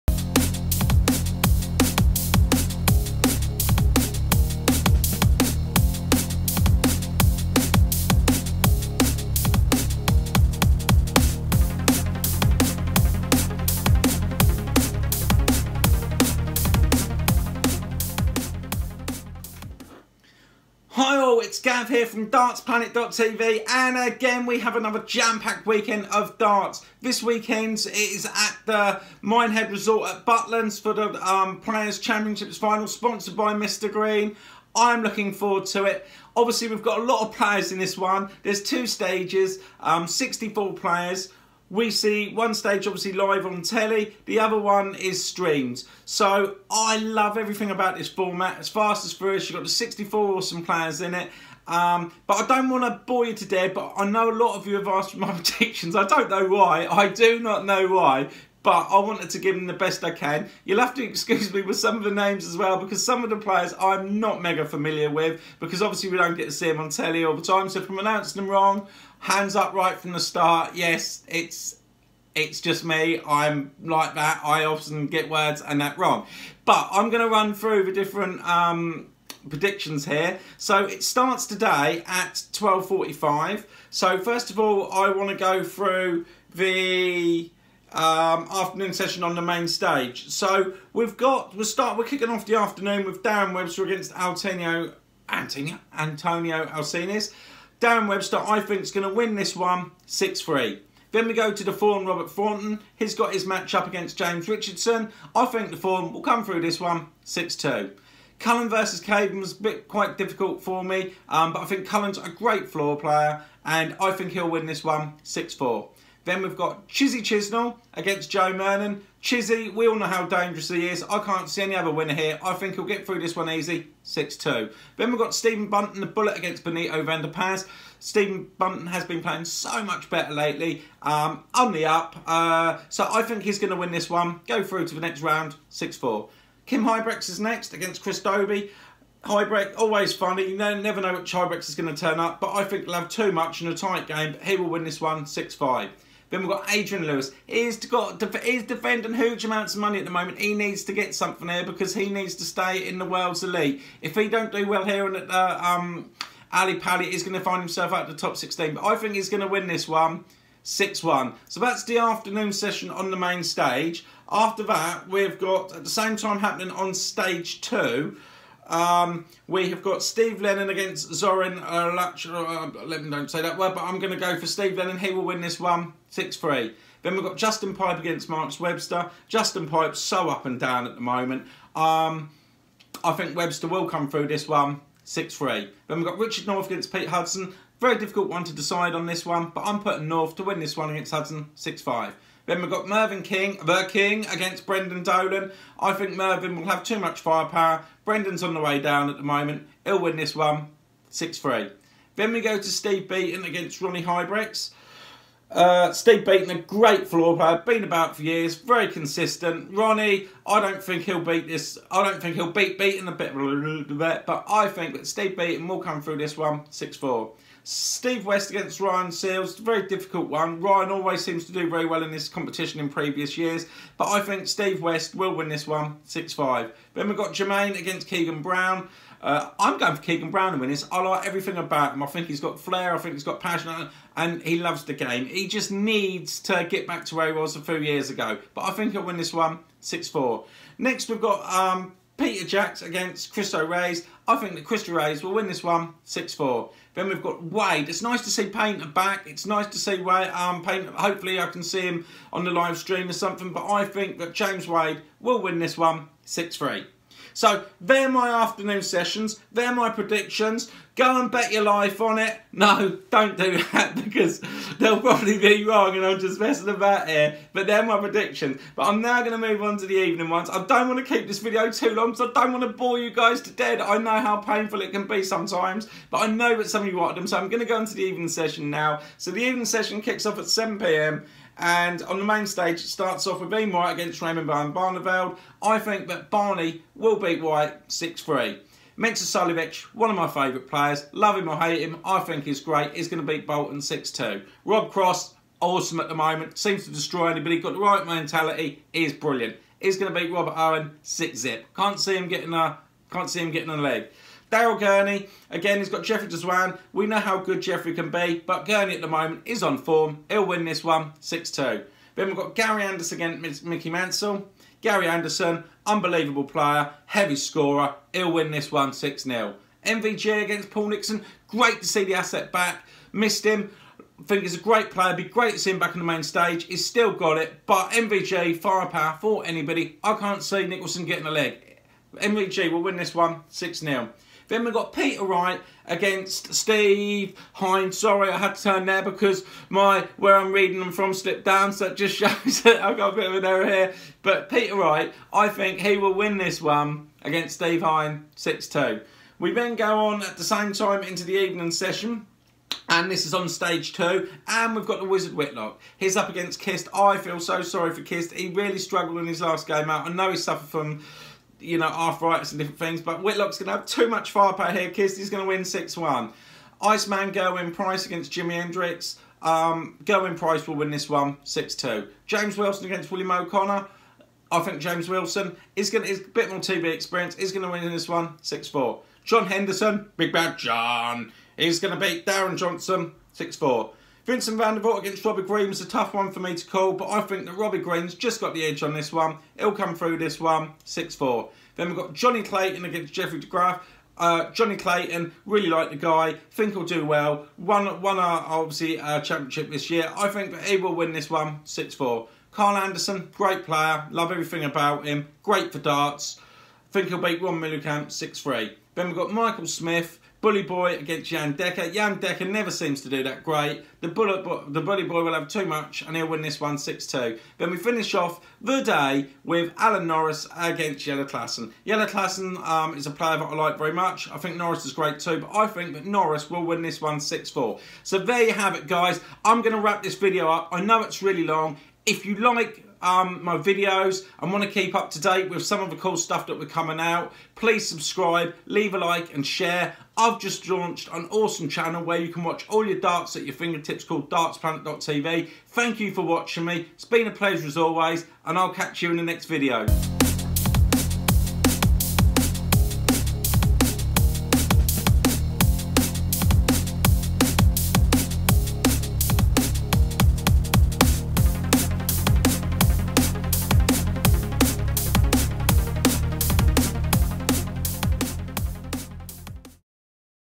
PREDICTIONS ROUND 1 OF THE DARTS PLAYERS CHAMPIONSHIP FINALS - MINEHEAD BUTLINS
These are my predictions for the 1st round of the Darts Players Championship finals in Mindhead Butlins.

I have been asked by many to do them, i have tried to keep this video as snappy as possible as their was lots to get through, hope you enjoy this video.